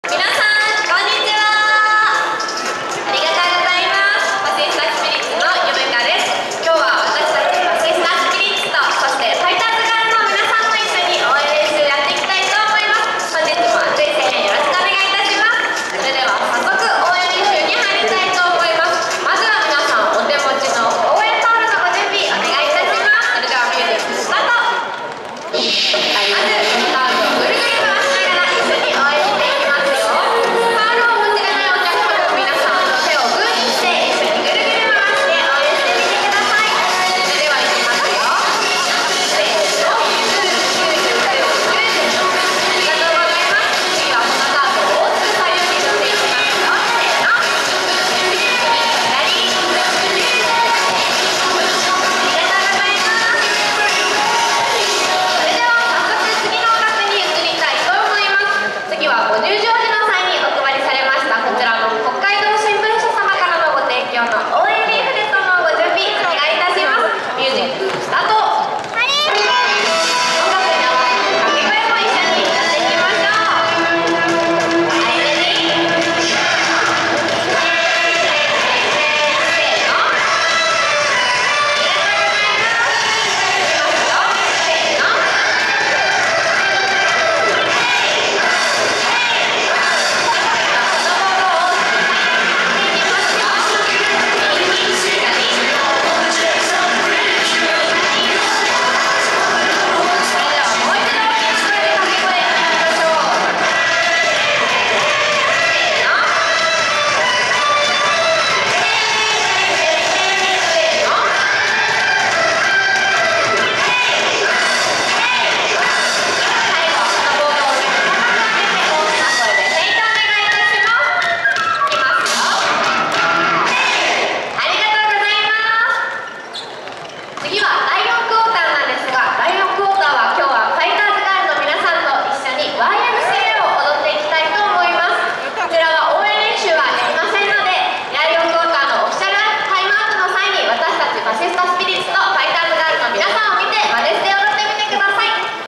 20200126 レバンガ北海道の応援レクチャーをするポリーちゃん
2020年1月26日、北海きたえーるでのレバンガ北海道のゲームは北海道日本ハムファイターズとのコラボデー！
ゲーム前の応援レクチャーをしているポリーです。